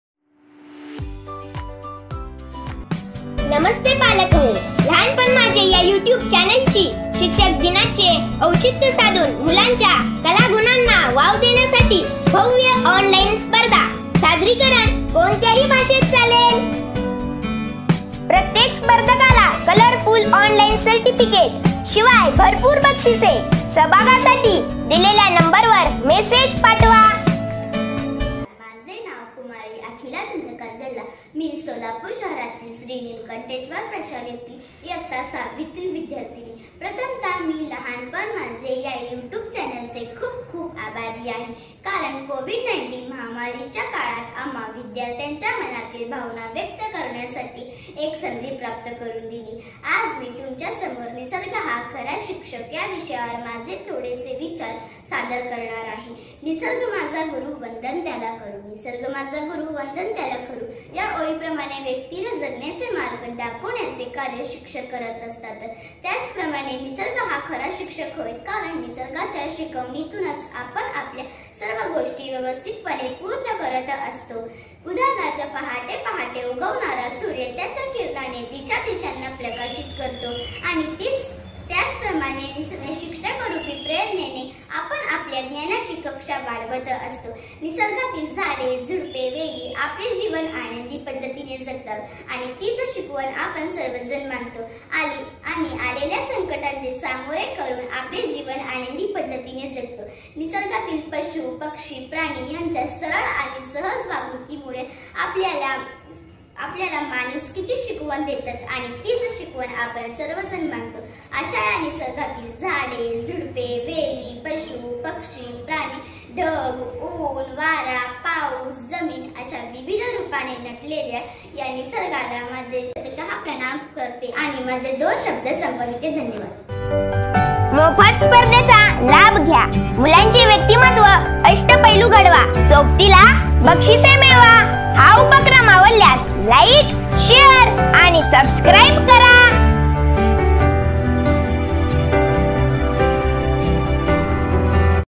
लहानपण माझे ll maze lahanpan ll भाषण ll स्पर्धक : अखिला चंद्रकांत जल्ला llस्पर्धक क्र.156 llगट 5 वा
विषय  :  भाषण        ll  निसर्ग खरा शिक्षक  ll
      स्पर्धकाचे नाव :  अखिला चंद्रकांत जल्ला